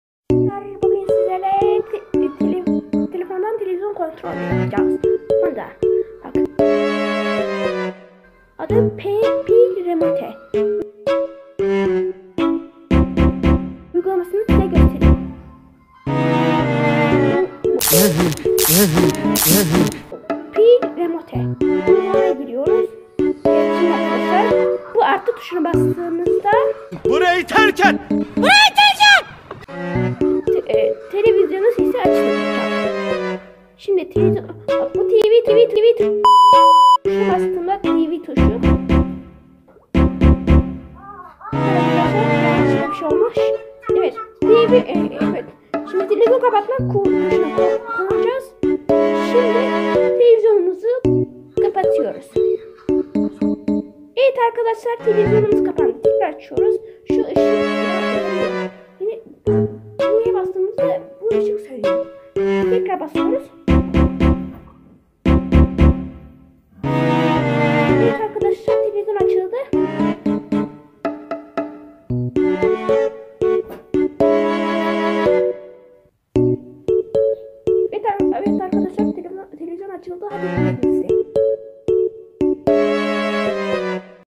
(Televizyonu Telefondan Kontrol Etmek) KOMİK MONTAJ🤣
Merhabalar Bugün (Televizyonu Telefondan Kontrol Etmek) Komik Montaj Videosu İle Karşınızdayım

Türkiye TEK YÜREK❤️